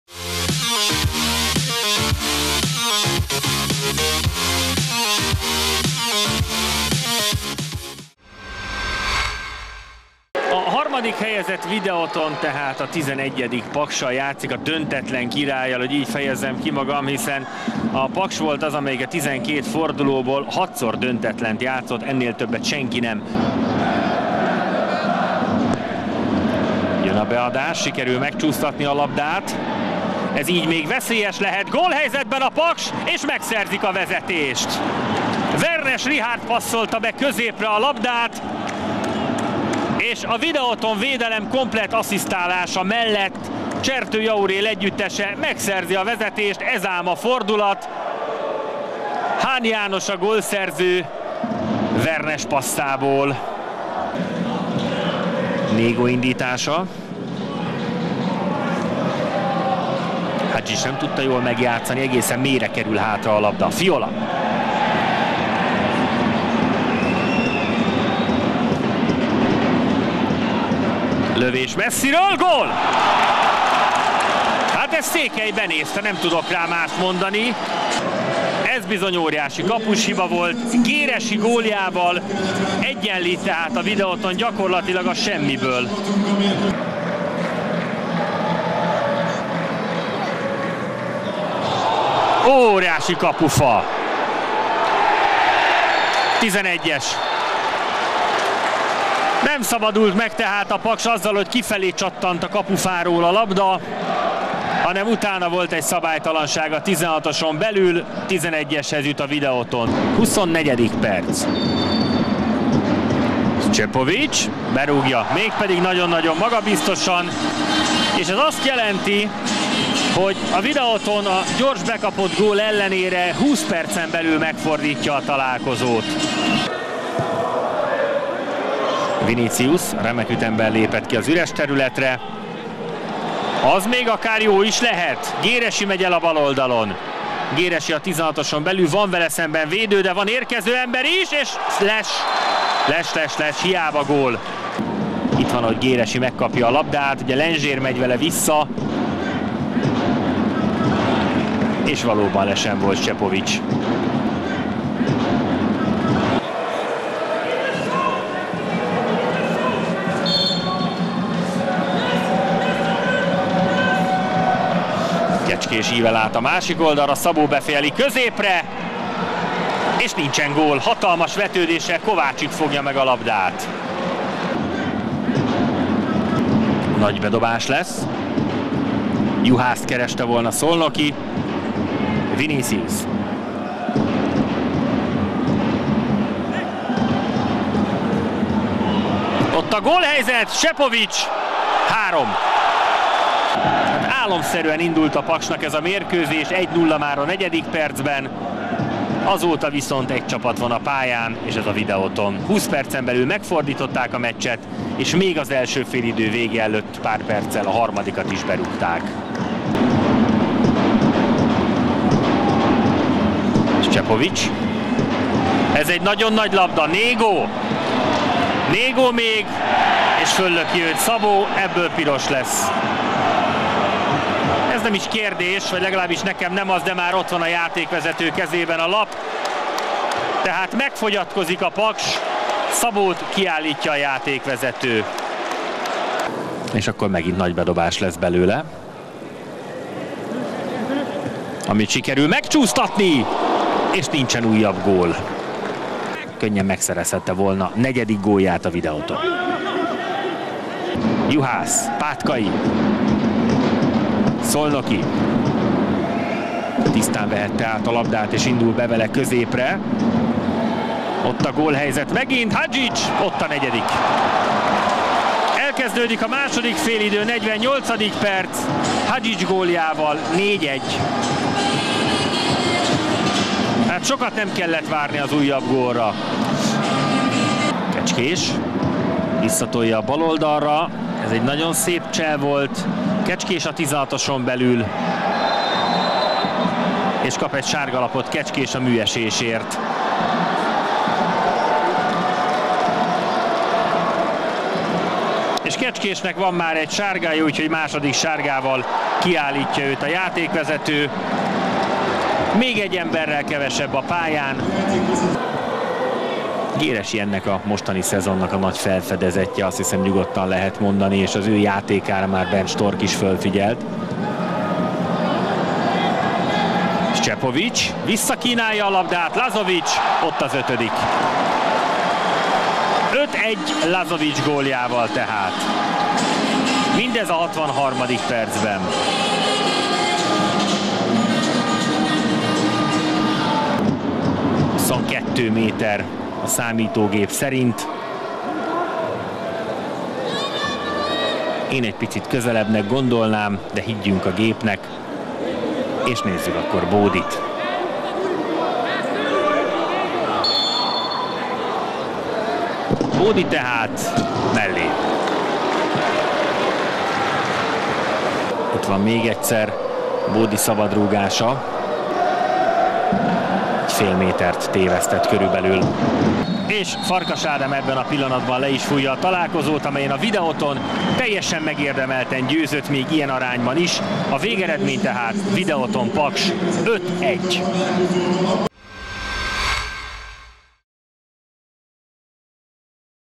Videoton FC - Paksi FC | 5-1 | OTP Bank Liga | 13. forduló | MLSZTV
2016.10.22., OTP Bank Liga 13. forduló
Videoton FC - Paksi FC 5-1 (3-1)
összefoglaló